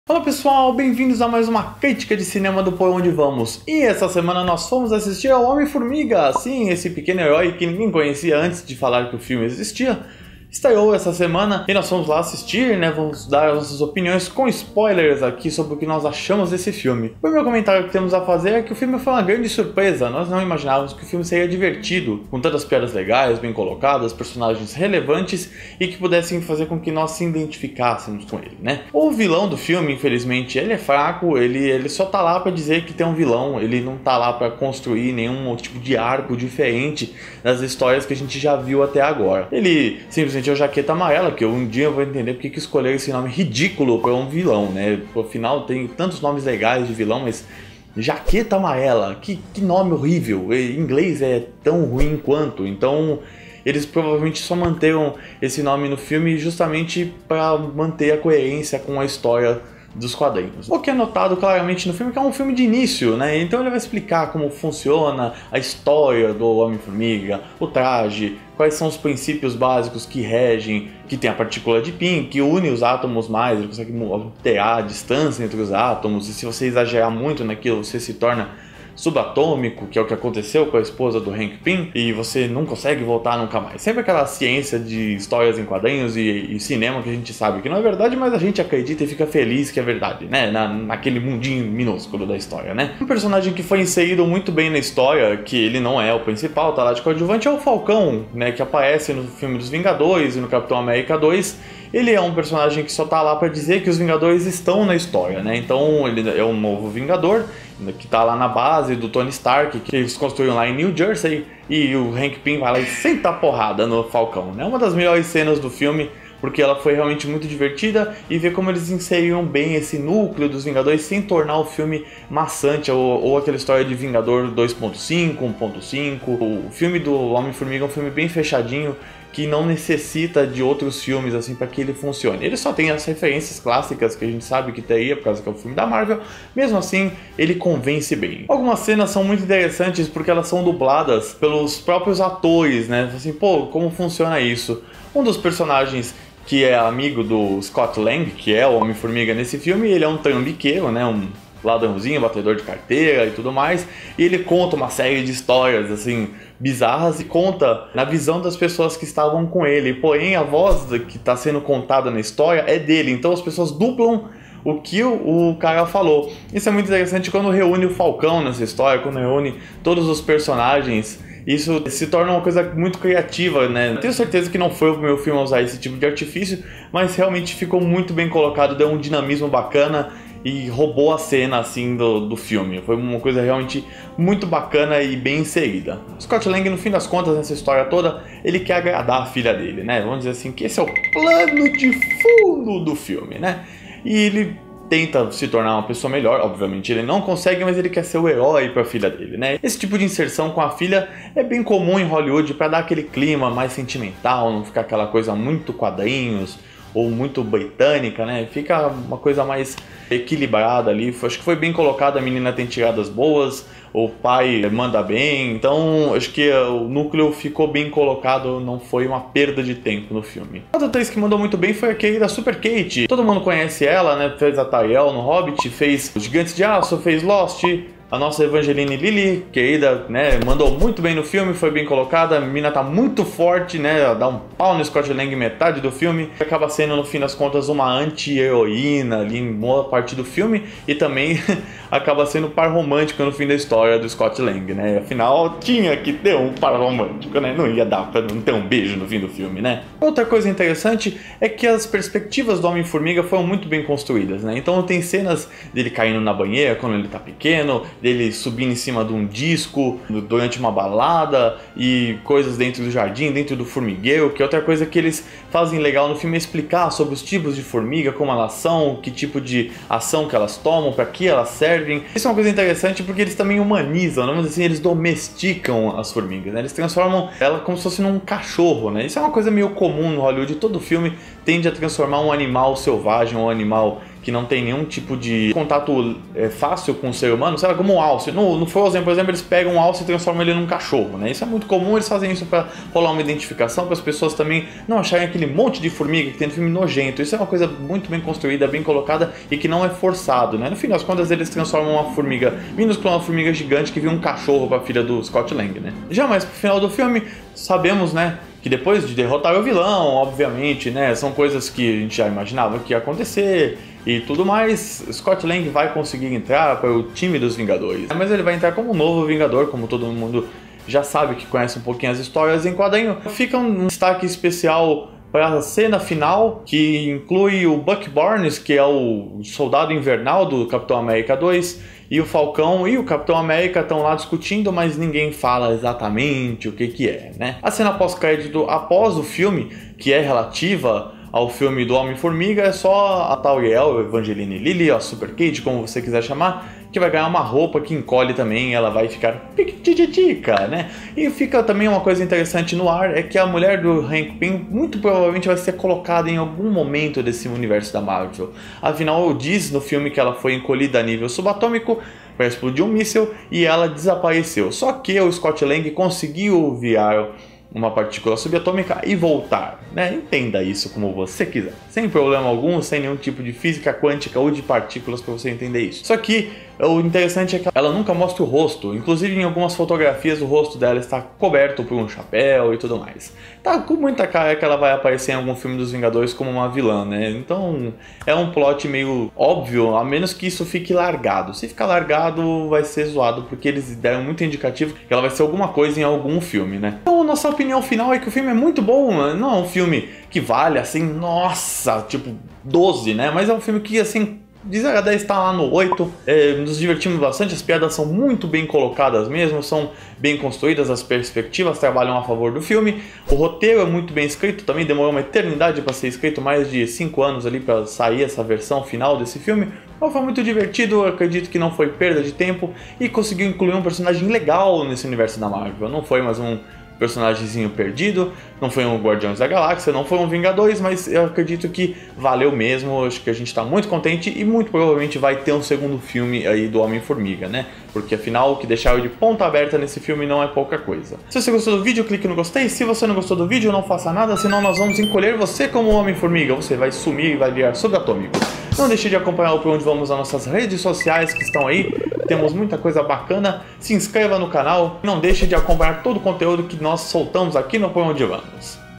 Crítica Homem-Formiga Com Spoilers
Esta no ar a critica de cinema do Por Onde Vamos! Nesta semana fomos assistir a Homem-Formiga, então confira nossas opiniões sobre o filme, saiba mais sobre os personagens e fique por dentro das cenas pós crédito!

Acompanhe-nos no Vine e Snapchat: @porondevamos
Veja nossas fotos no Instagram: @porondevamos